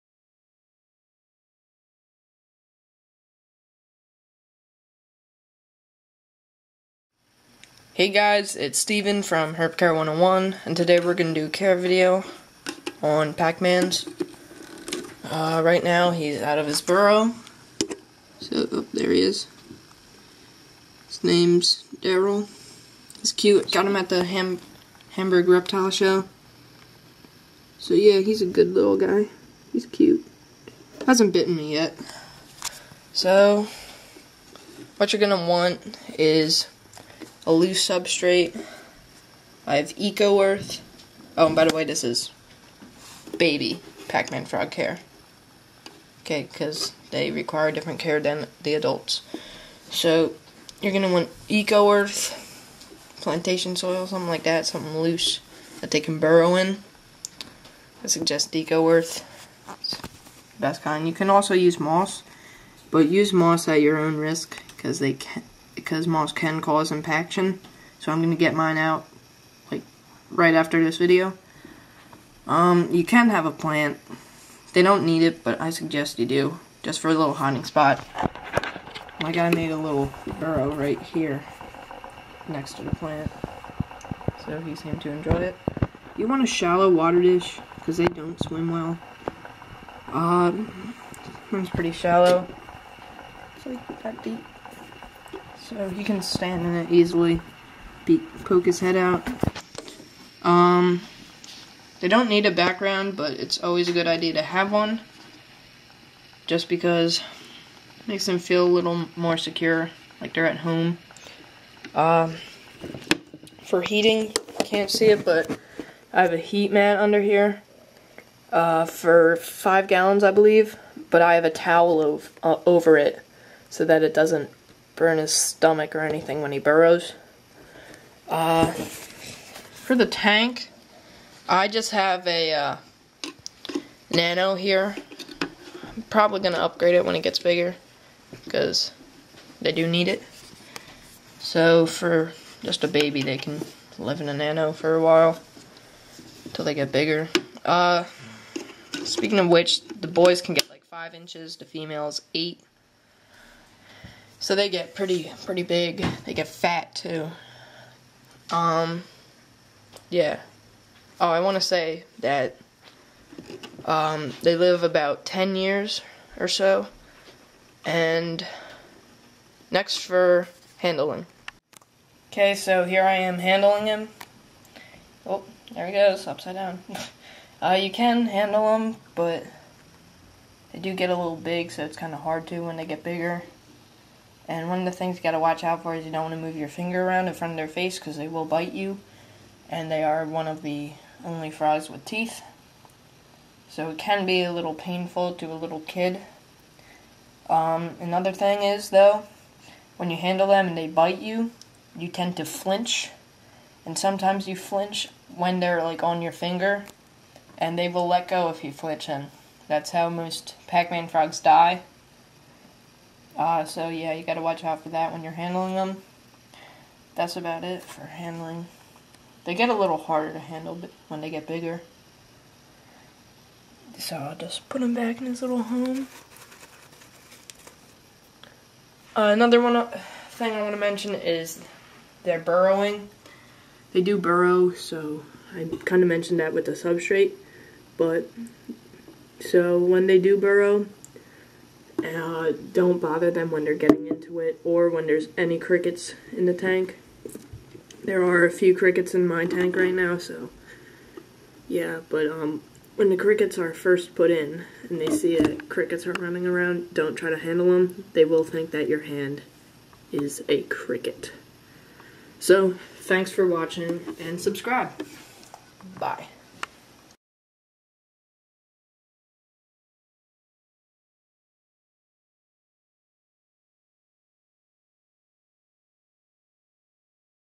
Pacman Frog Care
This video will show you how to take care of pacman frogs. This pacman frog is a baby, so his requirements are different to those of an adult.